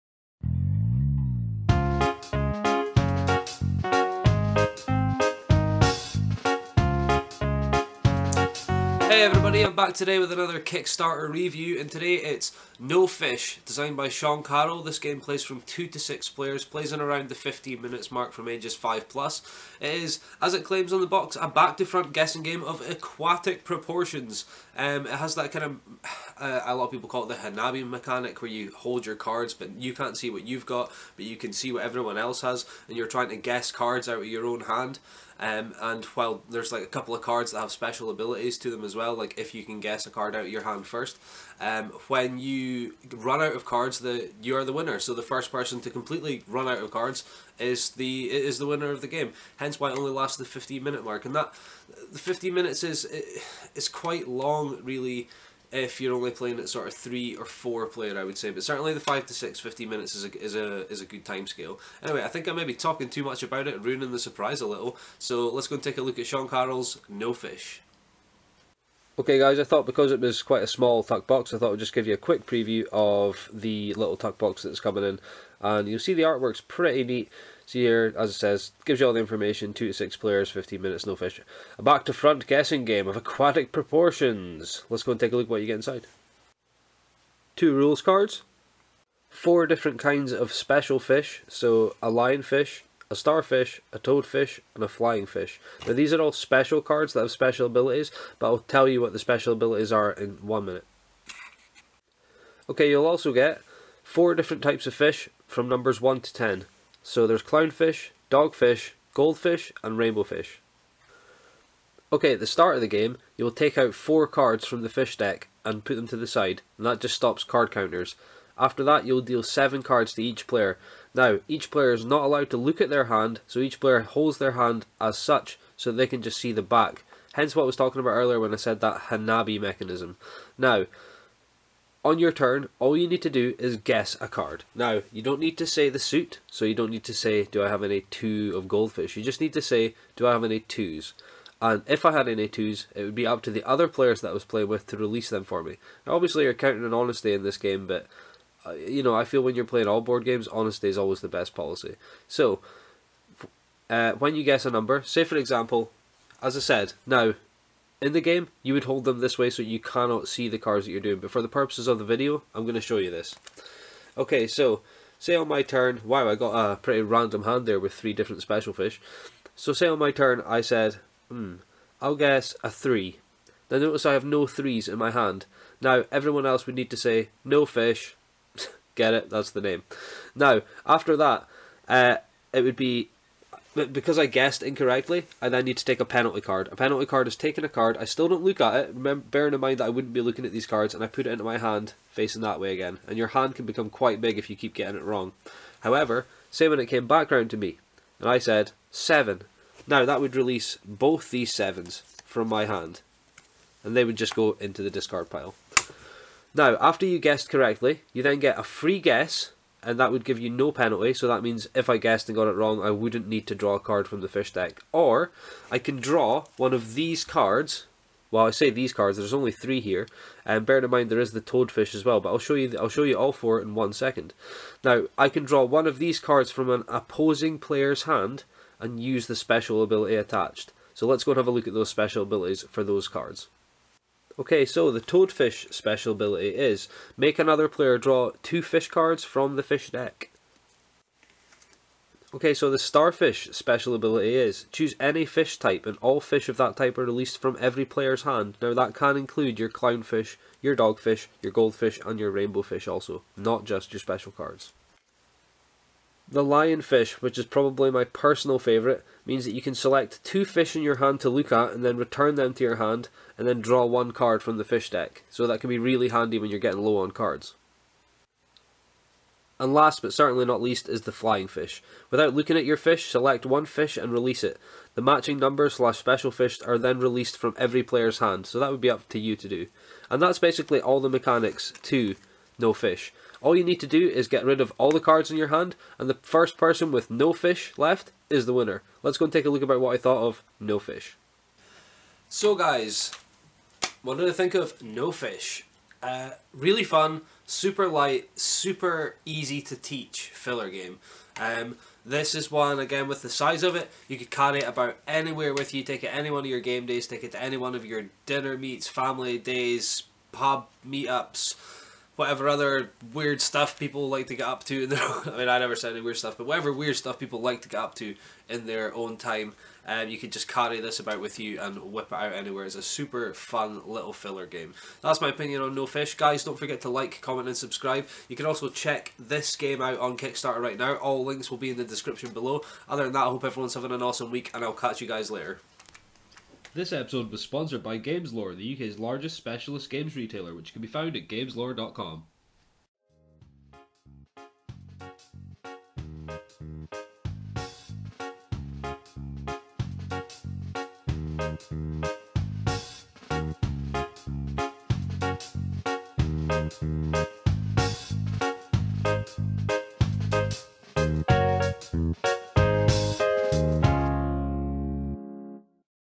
No Fish! - Kickstarter Review
This is a Kickstarter game designed by Sean Carroll, its a guessing game that requires a little thought. Check this out its No Fish.

and my links as usual.